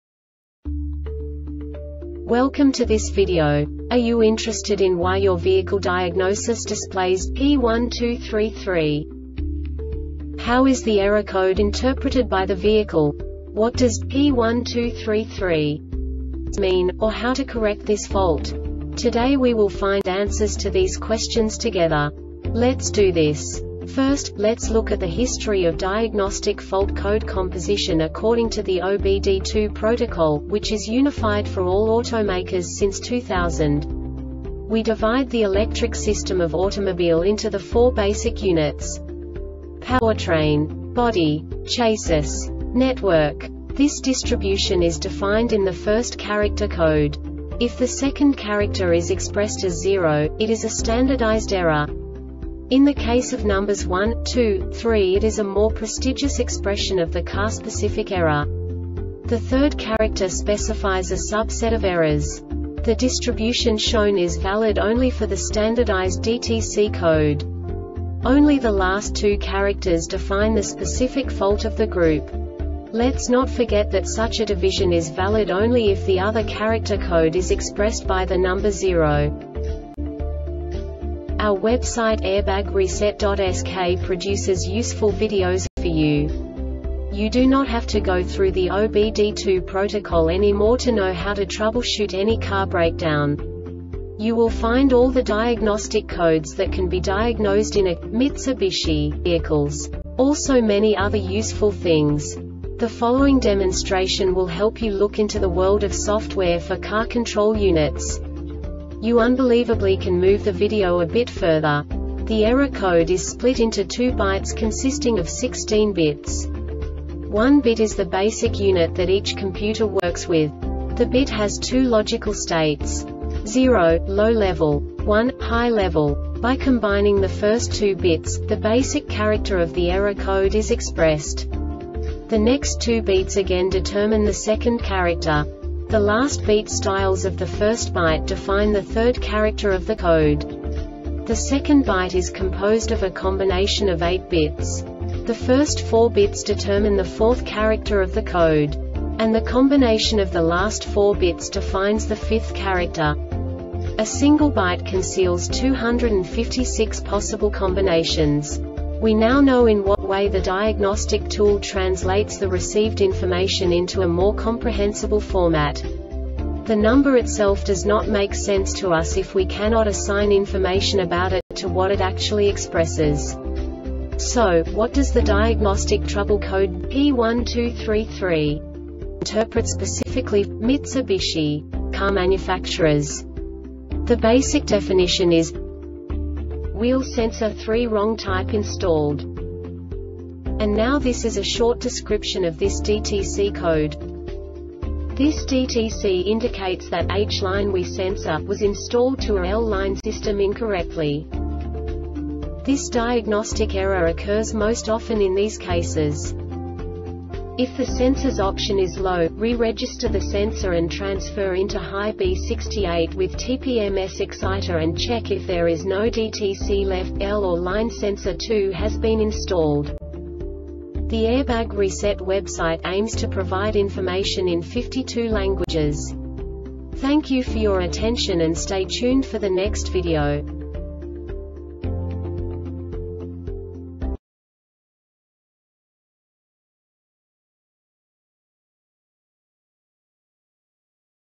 DTC Mitsubishi P1233 Short Explanation
Contents:
0:21 Basic DTC analysis according to OBD2 protocol standard.
1:48 Insight into programming
2:58 A diagnostically understandable description of the Diagnostic Trouble Code
3:05 Basic error definition

Make:
Mitsubishi
Code:
P1233
Definition:
Wheel Sensor 3 Wrong Type Installed
Description:
This DTC indicates that “H or Line WE sensor” was installed to a L or Line system incorrectly.
Cause:
 If the sensors option is LOW,Re-register the sensor and transfer into HIGH B68 with TPMS Exciter and check if there is no DTC left. L or Line Sensor 2 has been Installed